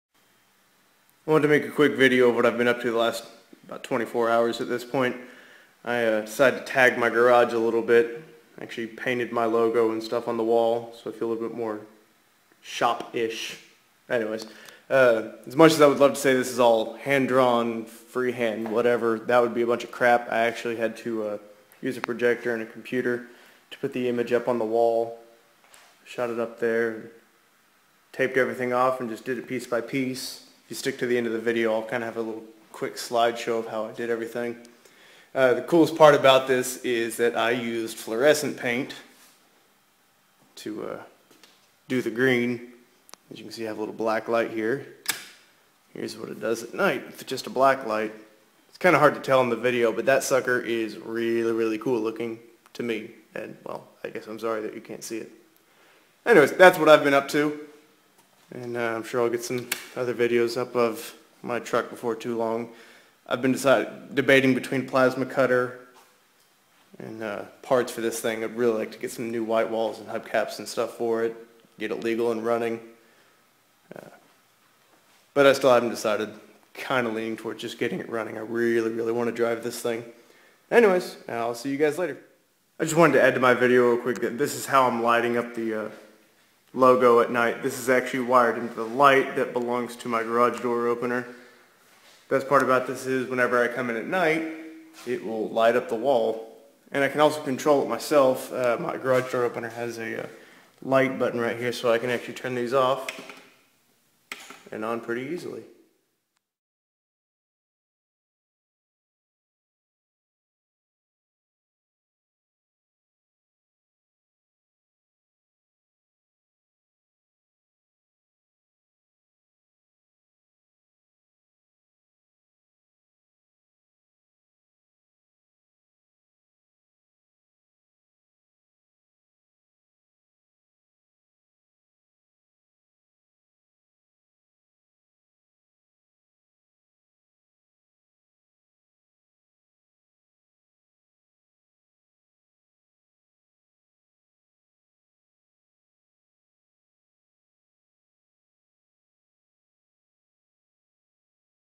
Florescent Garage Art with Black Light
I tagged my logo on the wall of my shop.  I'm pretty happy with it.